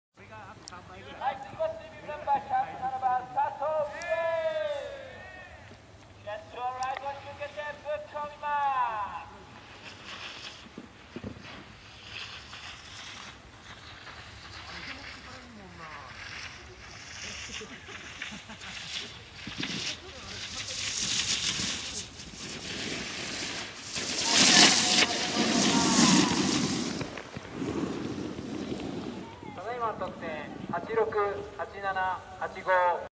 第5回マイアテク選 bibNo.107 フリーライディング
2015年03月29日 第5回マイアスノーボードテクニカル選手権

フリーライディング
 86 87 85＝258　　6位